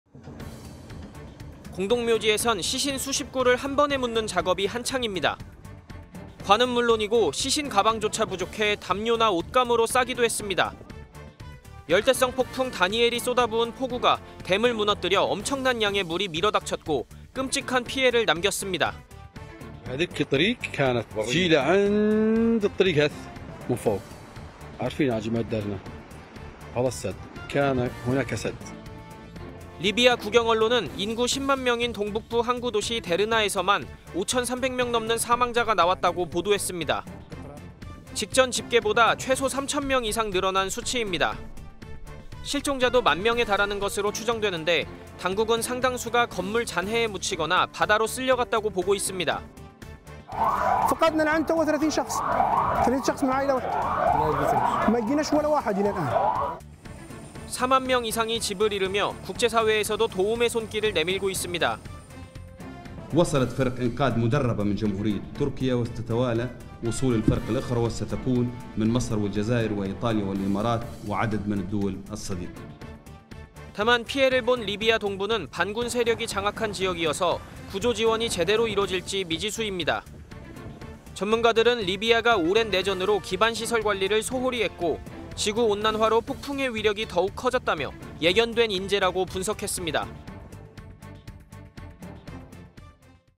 [자막뉴스] ＂시신이 쌓여있어요＂ 참혹한 리비아 상황, 무슨 일이? | 이슈픽
북아프리카 리비아를 할퀴고 간 홍수의 흔적은 참혹합니다.

목숨을 잃은 사람이 5천300명을 넘는데, 직전 집계보다 약 3천 명이나 늘어난 겁니다.

거리에는 시신이 즐비하고, 사망자 신원을 확인해줄 생존자가 부족하다는 말까지 나옵니다.